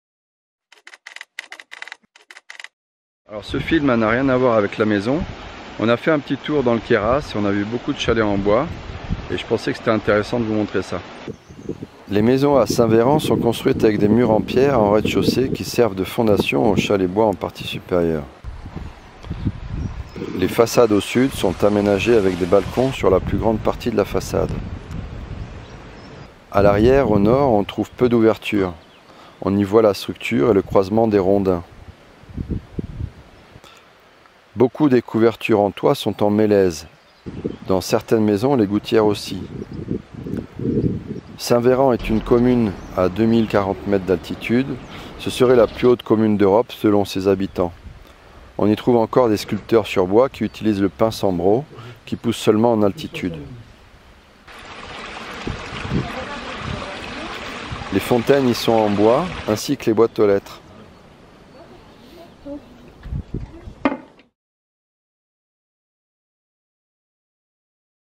Le Chalet du Queyras - Queyras loghouse
Construire un chalet fait main en bois massif en France est un vrai parcours du combattant. Il semble que tout soit fait pour nous détourner de ce mode de construction simple, performant et naturel, pourtant si prisé chez nos voisins slaves, et ce en faveur de ces constructions en ossature bois ou pseudo bois massif qui présentent pourtant de multiples inconvénients. L'industrie de la construction n'a bien sur aucun intérêt a développer une construction simple qui utilise presque un seul materiau polyvalent, mais nous si !  Building a hand made solid wood log house in France is a real obstacle course. It seems that all is done to distract us from this simple, natural and performant construction mode yet so prized in our slavic neighbours, this in favor to those framed house construction or pseudo solid wood houses which have many disadvantages. The construction industry has of course no interest to develop such easy construction which uses only one polyvalent material, but we do !